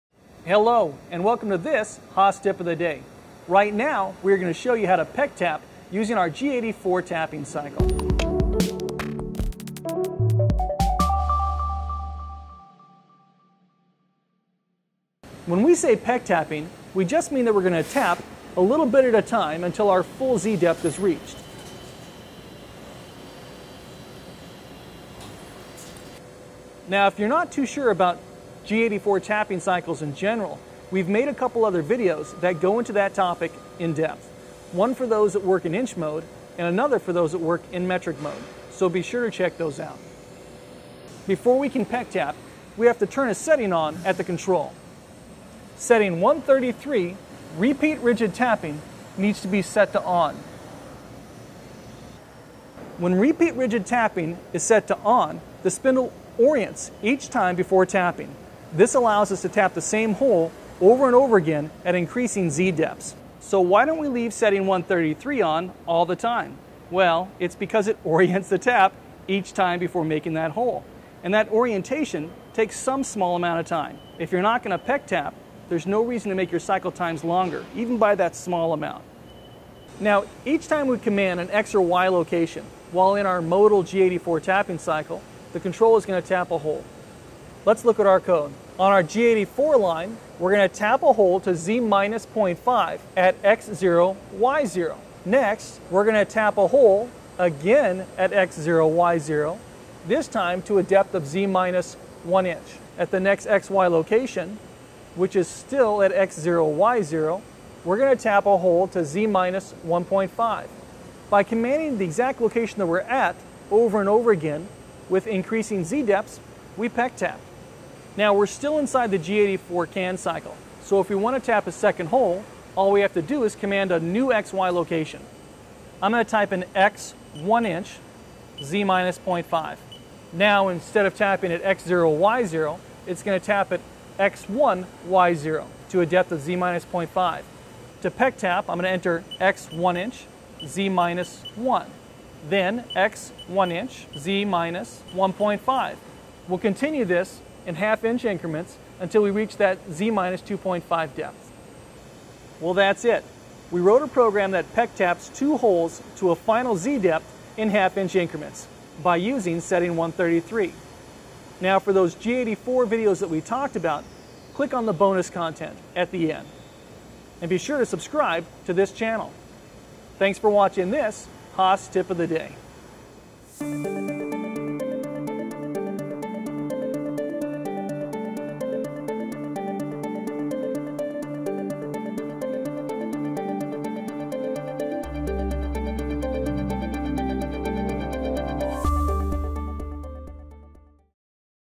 Simple Peck Tapping Using a G84 Tapping Cycle – Haas Automation Tip of the Day
Today, Mark shows you how to write a peck tapping program using G84, without using a CAM system! Mark also explains how to use Setting 133 . . . and when NOT to use it to keep your cycle times to a minimum.

Don't miss any "TIP OF THE DAY" videos. to join our email list!